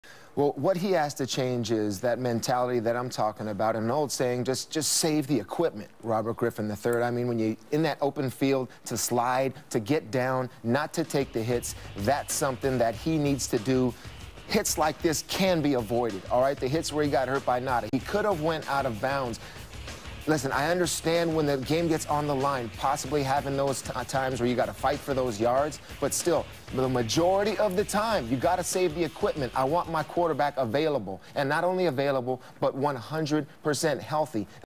RG3's "Greatest Hits" - Rookie Edition
They at least had the mercy to leave out the Haloti Ngata dump trucking, but STILL... damn, son. We need ya! Like Tedi Bruschi said: "Save the equipment..."